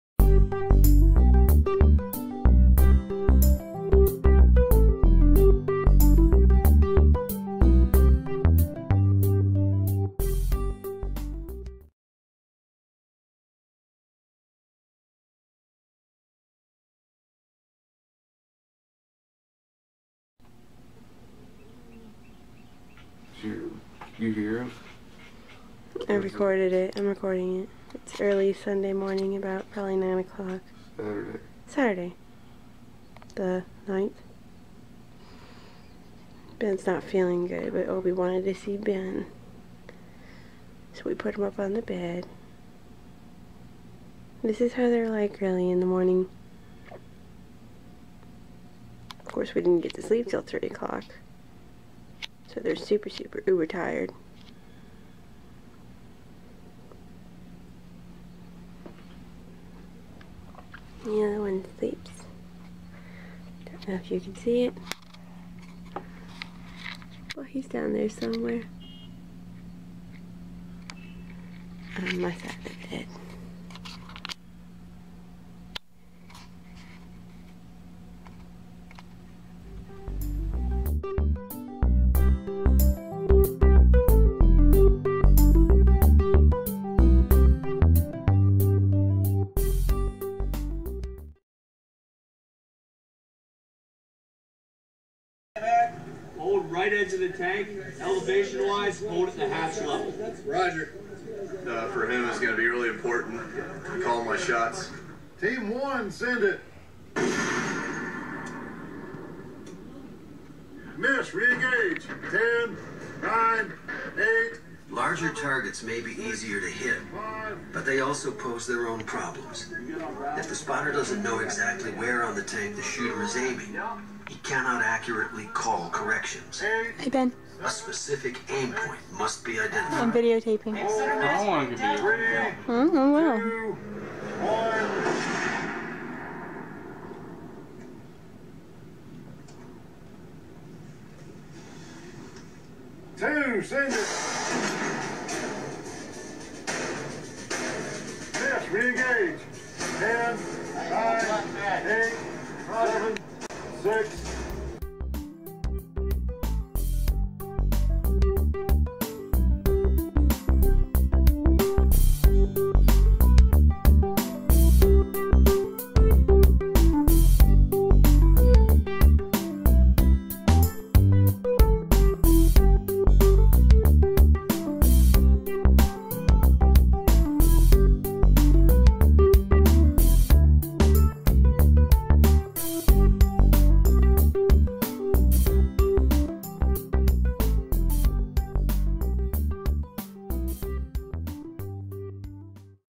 Adventures of Vader and Obi Wan Episode 2
Here are the many clips I have of Obi-Wan sleeping. I try to take as many as I can right when he is about to sleep, its hard to get him in that state when im around.
Obi is a mix between a Pomeranian  and Corgi and quite honestly one of the more unique looking dogs I have come across.